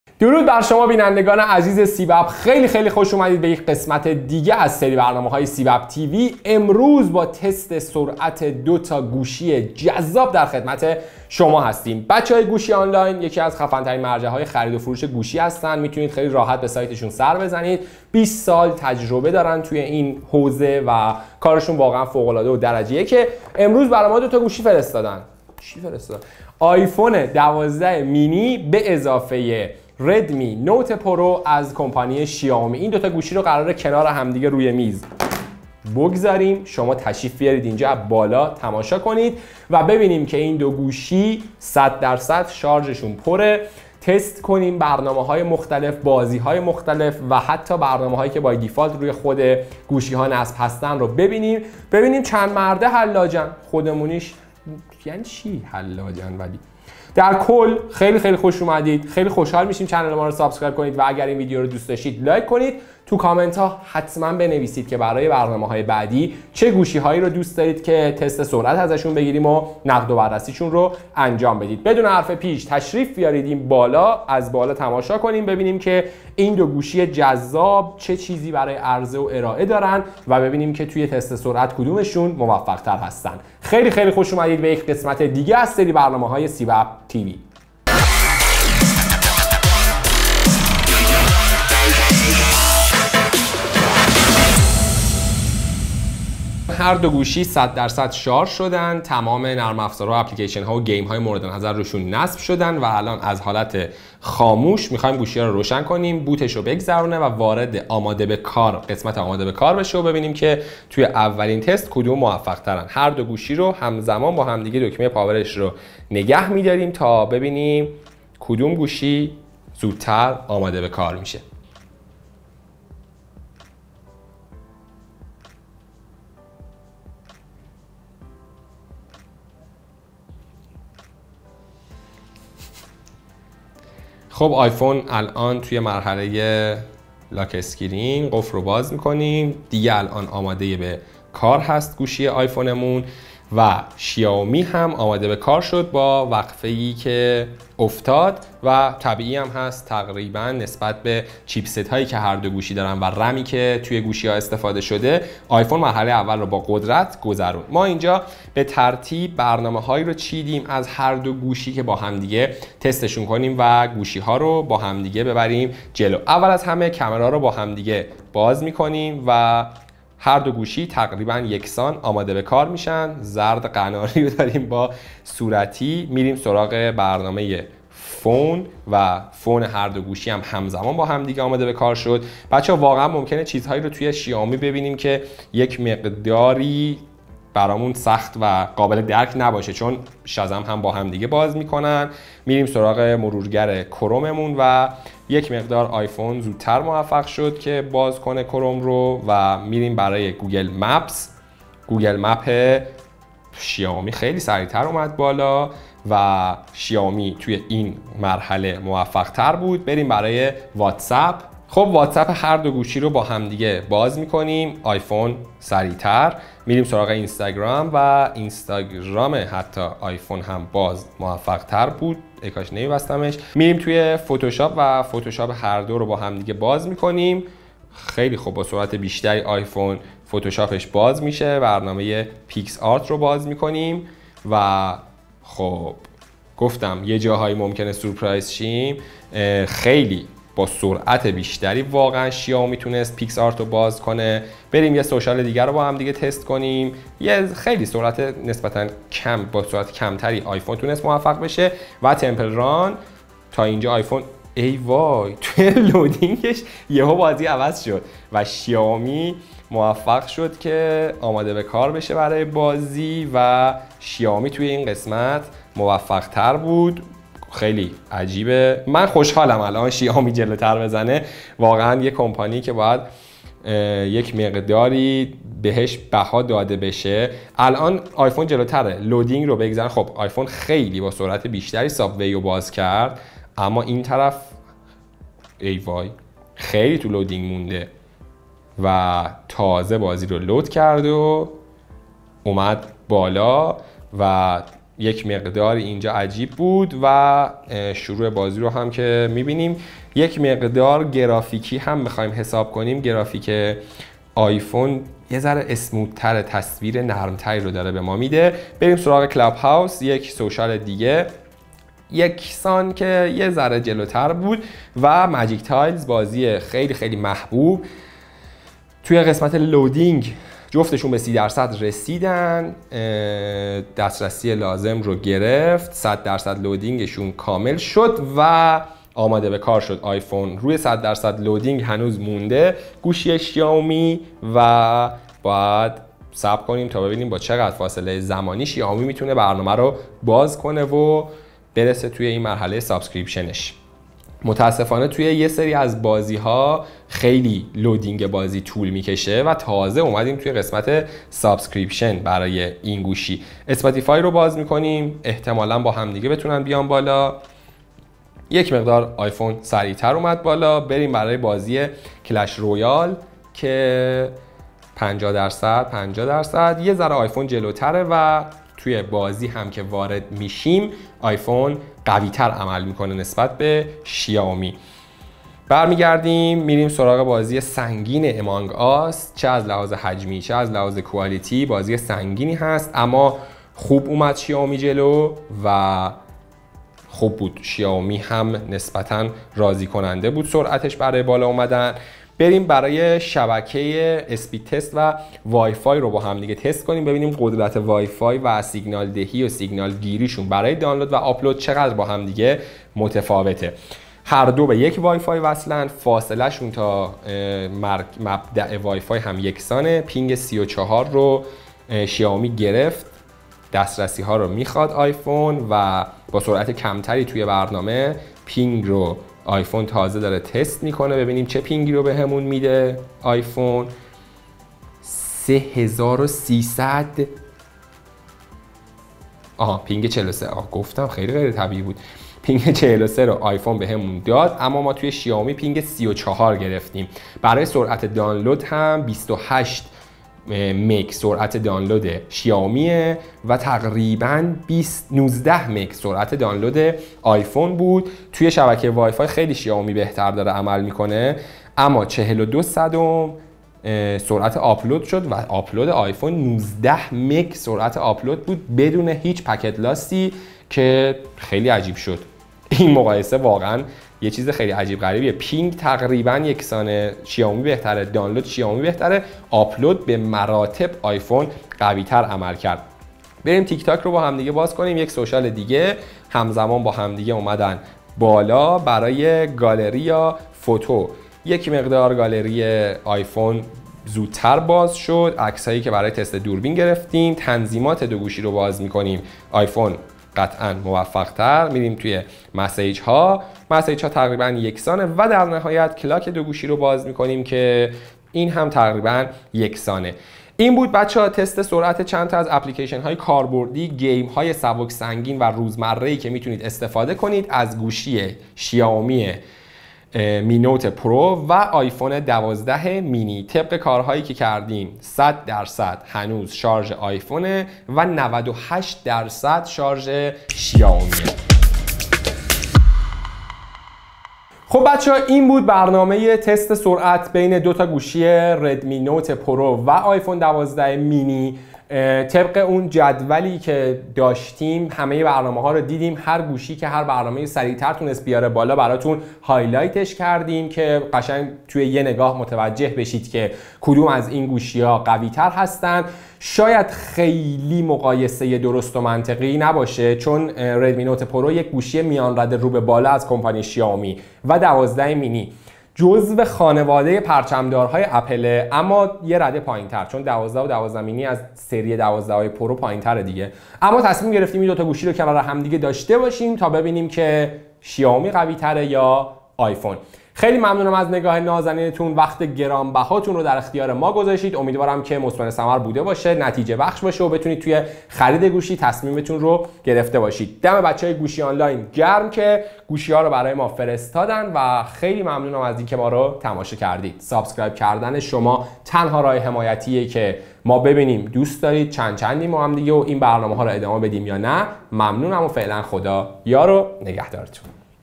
iPhone 12 Mini vs Redmi Note 10 Pro Speed Test | تست سرعت آیفون ۱۲ مینی و ردمی نوت 10 پرو شیائومی
در این ویدیو به  تست سرعت آیفون ۱۲ مینی و ردمی نوت 10 پرو شیائومی می‌پردازیم
iPhone 12 Mini vs Redmi Note 10 Pro Speed Test

سیب‌اپی‌های عزیز تو کامنت بگید به نظر شما کدوم گوشی عملکرد بهتری داشت؟ منتظر کامنت‌هاتون هستیم

اسپانسر:
مجموعه گوشی آنلاین مرجع قیمت موبایل ایران با بیش از ۲۰ سال سابقه در زمینه فروش تلفن همراه